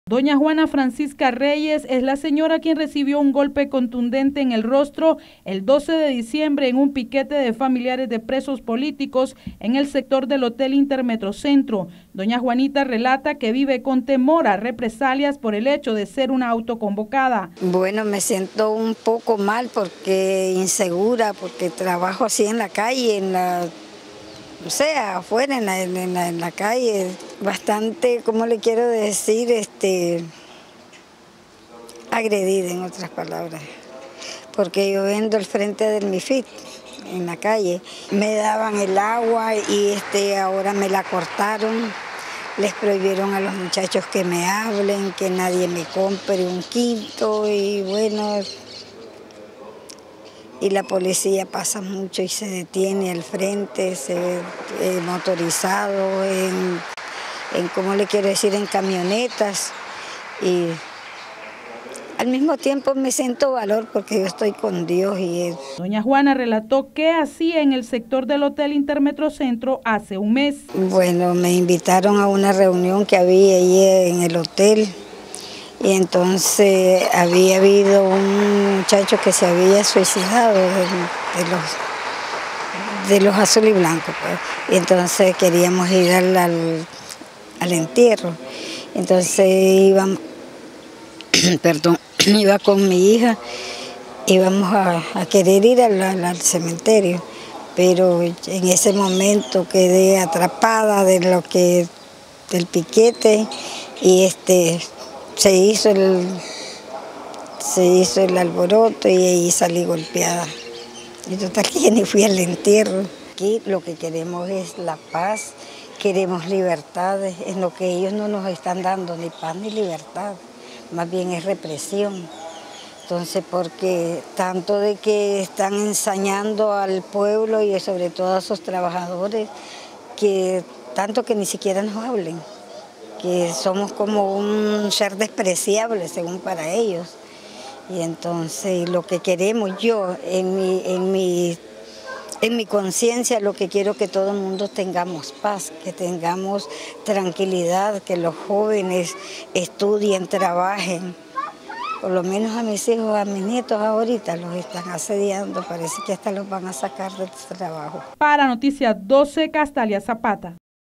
DOÑA JUANA REYES SIENTE TEMOR A REPRESALIAS
A un mes de resultar herida en un plantón, doña Juana Reyes siente temor a represalias.